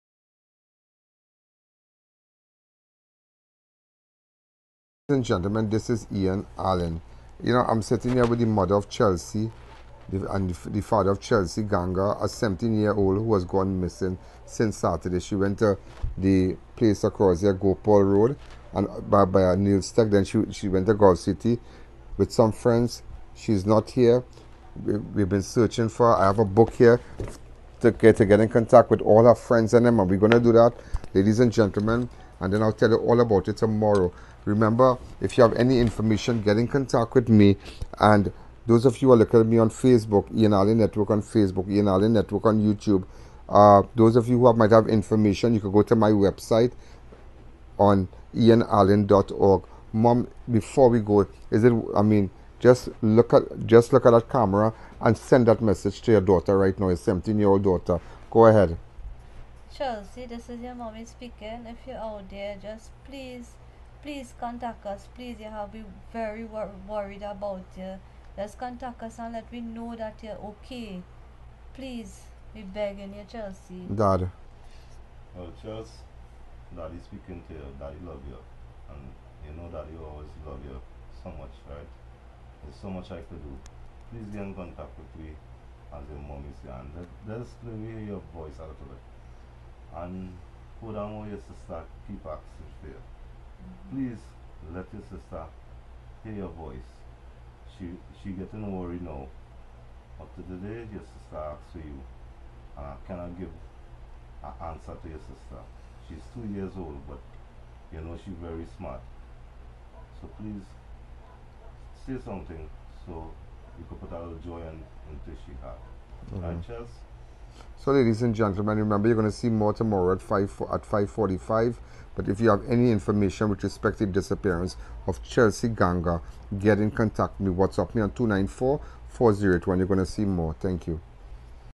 Parents of missing Chelsea Ganga,17, makes a public plea for the safe return on their daughter!!!
I just spoke to the parents of missing 17-year-old Chelsea Ganga as they made a public plea for the safe return of their daughter.

Chelsea Ganga, 17, of Diamond Village, San Fernando, was last seen on Saturday 29th April, 2023.

Be viewing Crime Watch today Wednesday 3rd May, 2023 @5:45pm for more !!!